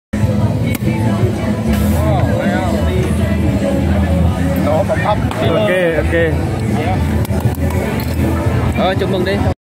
bạn Lào khui bia bằng nắp chai bia
bia lào